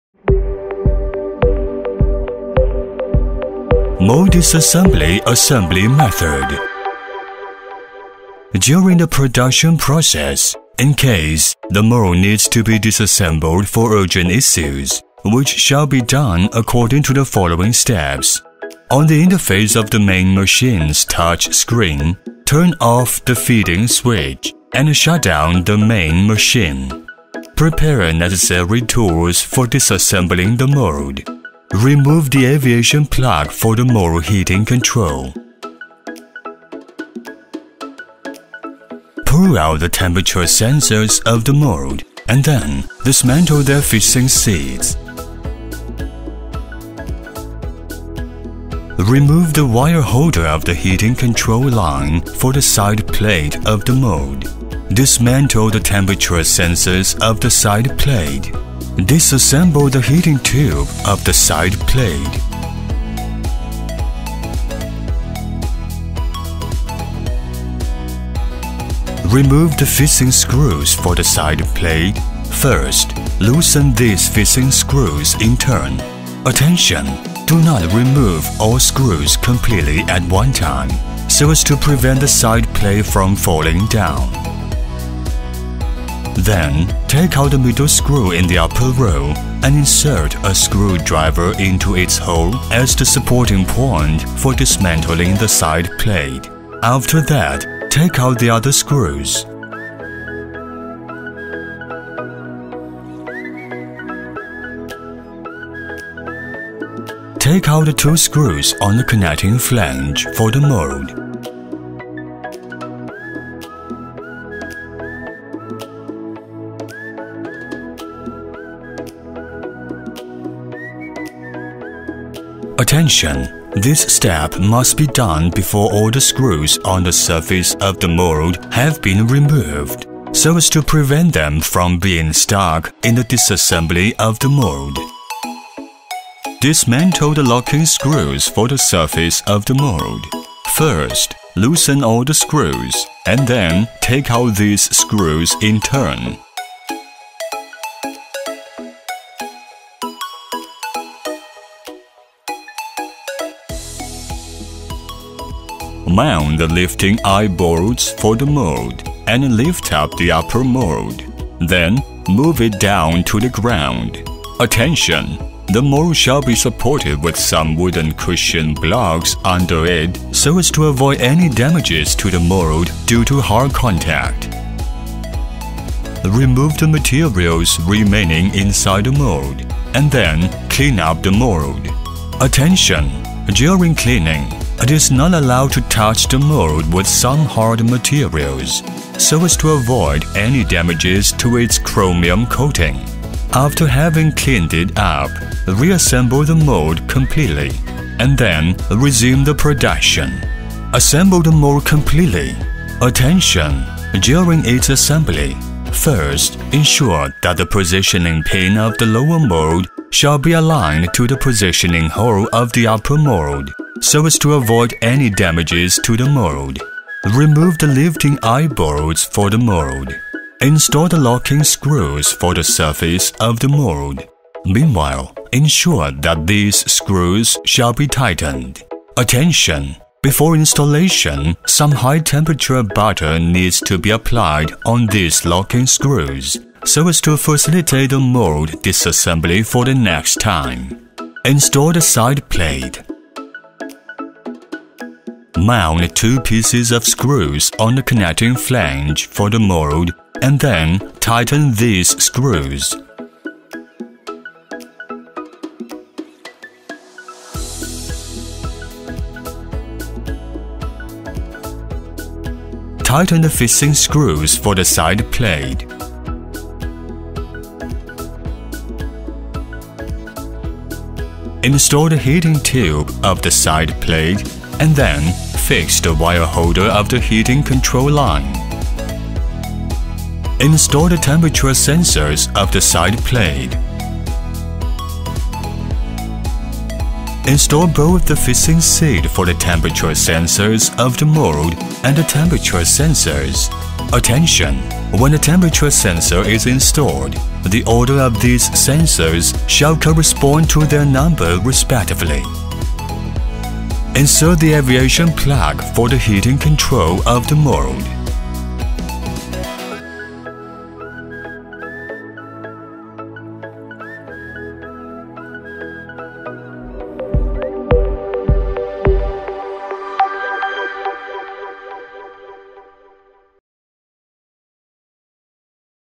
Boyu mould disassemble/assembly method
In this video, you can see the method of mould disassemble/assembly
In case the mould needs to be disassemble for urgent issues
Following steps:
1、Turn off the feeding switch and shut down the main machine
2、Prepare necessary tools for disassembling the mould
3、Remove the aviation plug
4、Pull out the temperature sensors
5、Dismantle fixing seats
6、Remove the wire holder of the heating control line
7、Disassemble the heating tube of the side plate
8、Dismantling the side plate
9、Loosen these fixing screws
10、Take out the middle screw in the upper row
11、Insert a screwdriver into its hole
12、Take out the two screws on the connecting flange for the mould
13、Diamantle the locking screws for the surface of the mould
14、Mount the lifting eyebolts for the mould
15、Remove the materials remaining inside the mould
16、Re-assemble the mould completely and then resume the production

ADD: No.188-6, xigan Road, Fangqian, Xinwu District, Wuxi, Jiangsu, China